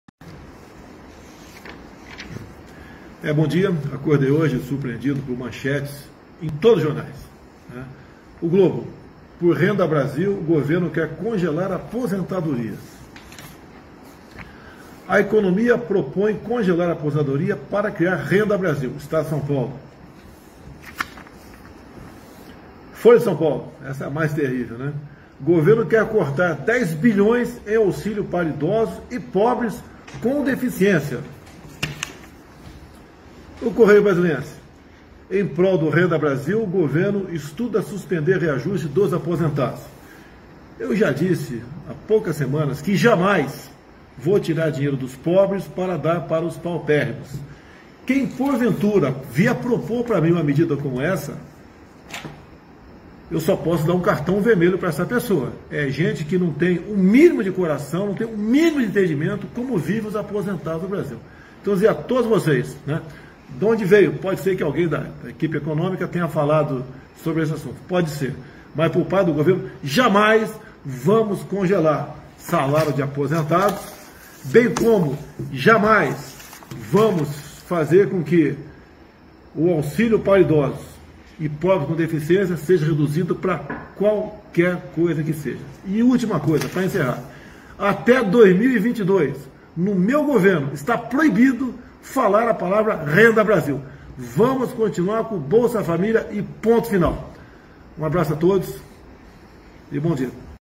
Bolsonaro se irrita com notícias sobre corte em aposentadorias
Presidente se irrita com a imprensa brasileira. Ele nega que programa social tenha objetivo de cortar renda de aposentados. Nega que economia tenha objetivo de avançar sobre renda de aposentados e pessoas com deficiência. Não haverá corte em aposentadorias e na renda de pessoas deficientes. Presidente desabafa em vídeo e diz que não se fala mais em Renda Brasil. O programa social do governo federal continuará se chamando Bolsa Família.